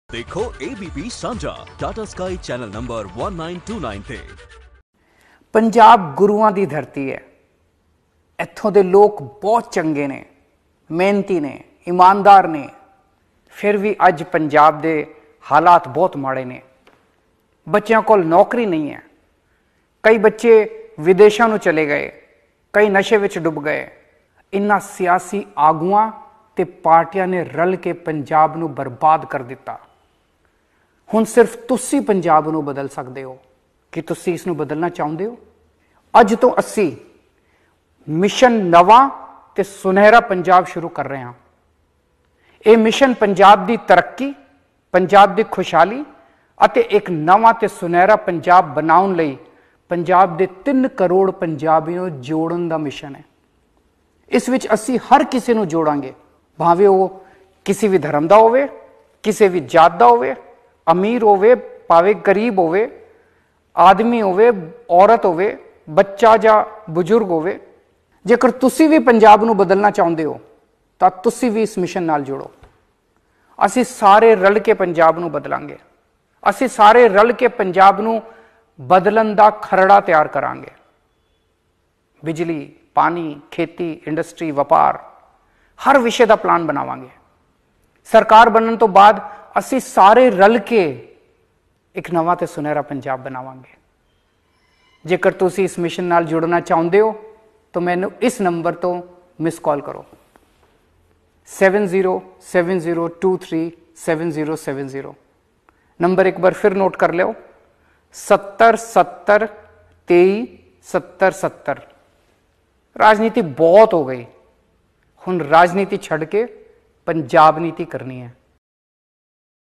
Arvind kejriwal ਨੇ ਸੁਨਹਿਰਾ ਪੰਜਾਬ' ਬਣਾਉਨ ਲਈ ਕੀਤੀ ਨਵੇਂ ਮਿਸ਼ਨ ਦੀ ਸ਼ੁਰੂਆਤ ! ABP Sanjha
ਕੇਜਰੀਵਾਲ ਨੇ ਸ਼ੁਰੂ ਕੀਤਾ 'ਮਿਸ਼ਨ ਨਵਾਂ ਤੇ ਸੁਨਹਿਰਾ ਪੰਜਾਬ'
ਮਿਸ਼ਨ ਵਿੱਚ ਪੰਜਾਬ ਦੇ 3 ਕਰੋੜ ਲੋਕਾਂ ਨੂੰ ਜੋੜਨ ਦਾ ਟੀਚਾ
ਪੰਜਾਬ ਦੀ ਤਰੱਕੀ ਤੇ ਖੁਸ਼ਹਾਲ ਬਣਾਉਣ ਦਾ ਮਿਸ਼ਨ- ਕੇਜਰੀਵਾਲ